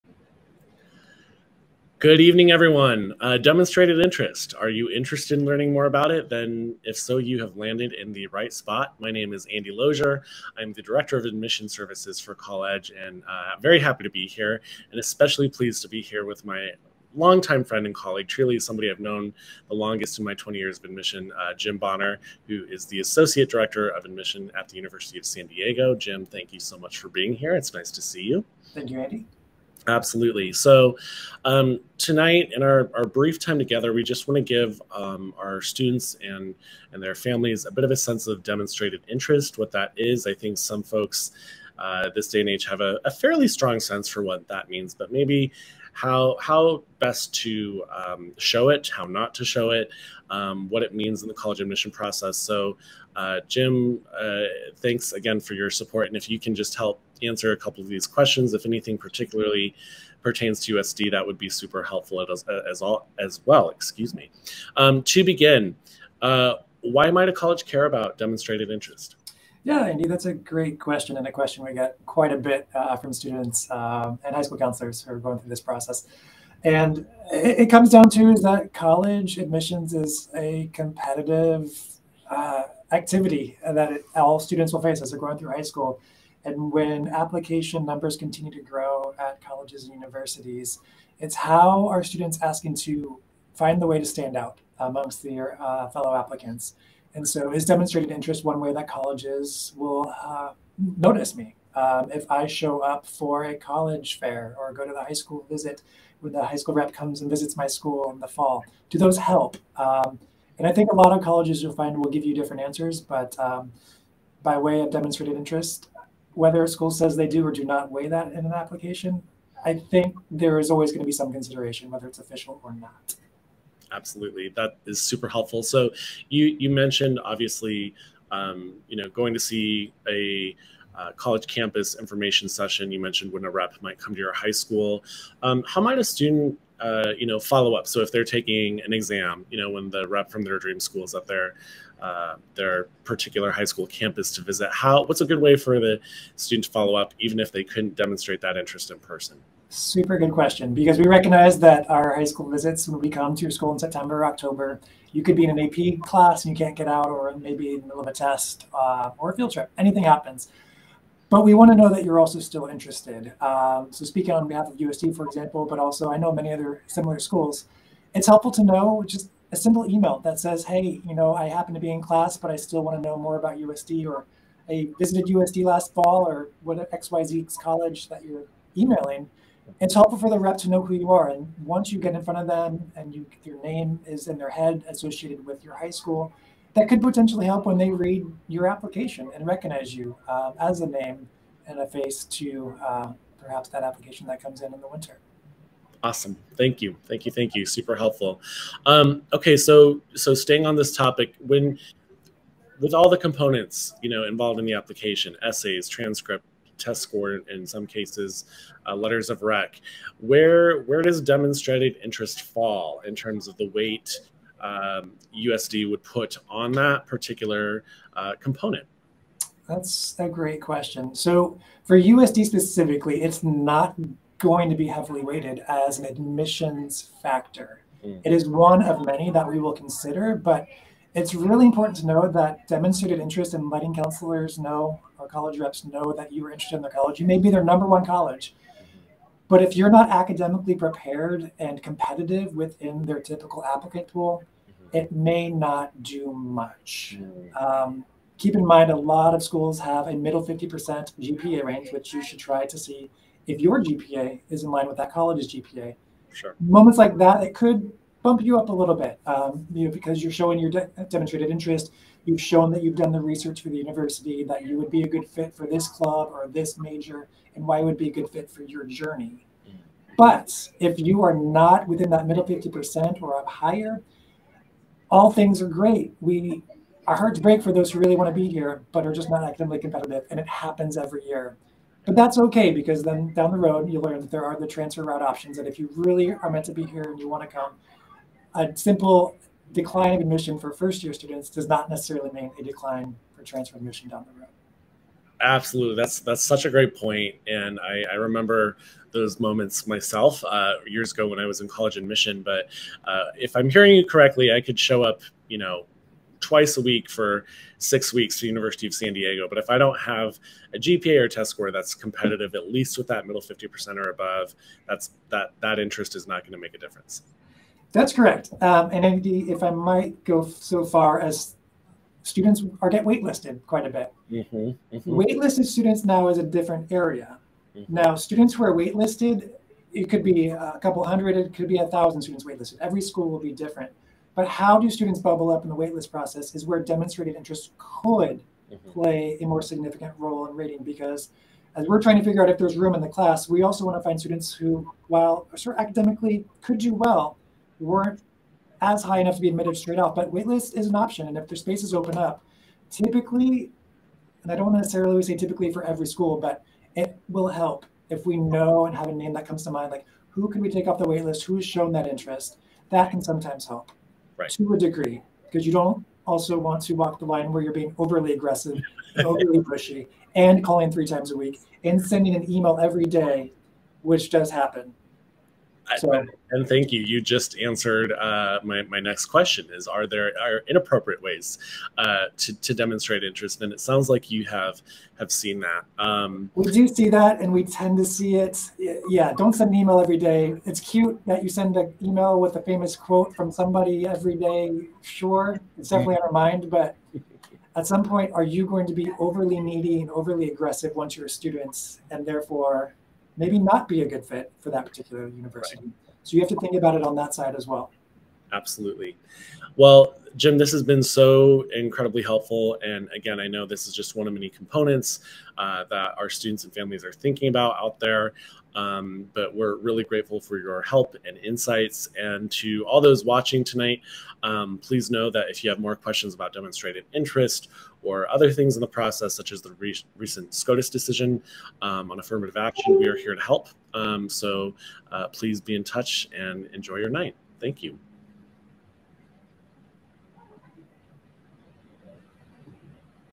Demonstrated Interest: How to show a college you're really into them
Colledge Director Andy Losier and Jim Bonner of University of San Diego dive into the nuances of why colleges care about Demonstrated Interest, who it helps and how to demonstrate it.